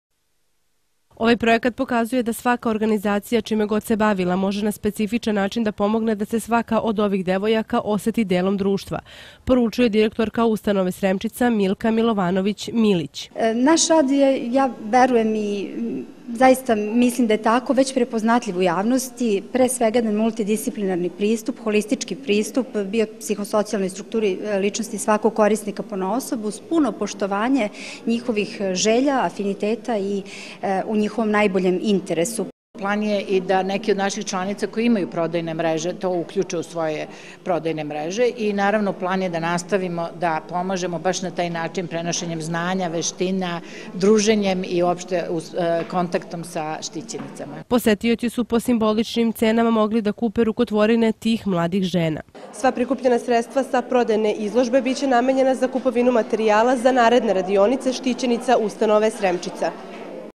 Dobrotvorni bazar za štićenice Doma "Sremčica"
6. mart 2014. -- TV Skay +:  Dobrotvorni bazar  za štićenice Doma "Sremčica"
Udruženje poslovnih žena Srbije je u sredu, 5. marta 2014. godine, u Beogradu organizovalo

Osmomartovsku dobrotvornu prodajnu izložbu radova štićenica Ustanove za decu i mlade „Sremčica". Posetioci bazara su po simboličnim cenama mogli da kupe rukotvorine i proizvode nastale u okviru projekta „Osposobljavanje mladih žena s telesnim i razvojnim smetnjama" i  radionicama kojima je rukovodila Marija Ivanković Jurišić, članica UPŽ Srbije i vlasnica modne radionice „Marija Hand Made" iz Šapca.
Projekat je realizovan u saradnji sa Međunarodnim klubom žena (IWC).
Sagovornice u prilogu su Olivera Popović, potpredsednica UPŽ Srbije i Milka Milovanović Minić, direktorka Ustanove za decu i mlade "Sremčica"